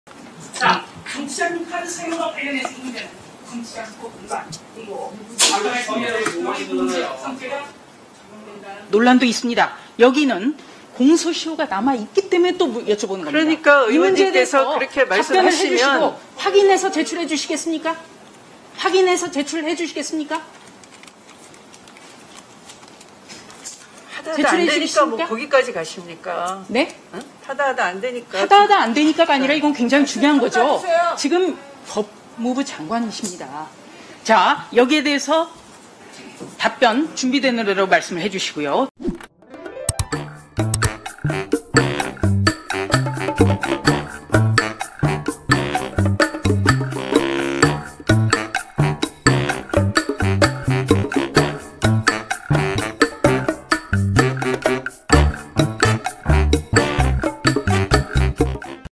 추미애 "하다하다안되니깐" 논점 흐리기?...조수진은 아랑곳 않고 계속된 추궁
9월 21일 열린 국회 법사위 전체회으에서 조수진 국민의힘 의원이 추미애 법무부 장관에게 질의하고 있다.